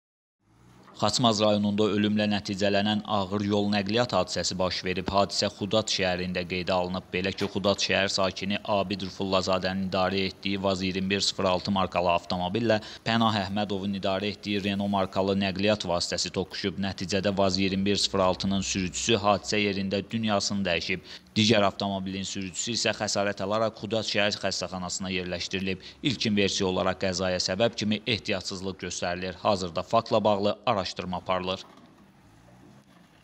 Yol qəzası ölümlə nəticələndi
İlkin versiya olaraq qəzaya səbəb kimi ehtiyatsızlıq göstərilir